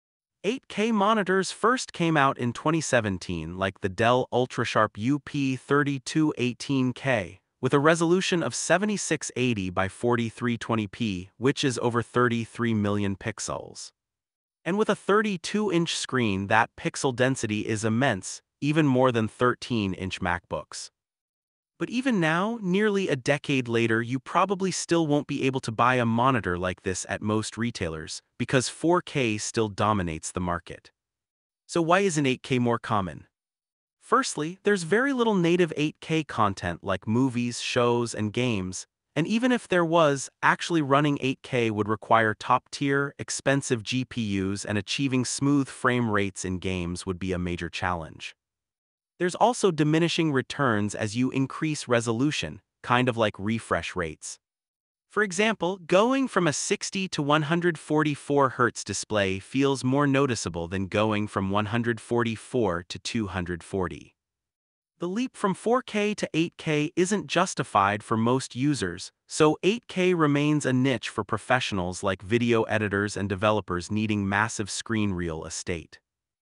Why 8K Monitors Still Isn't Mainstream (after nearly a decade)
In this video we discuss why 8K monitors still aren't common.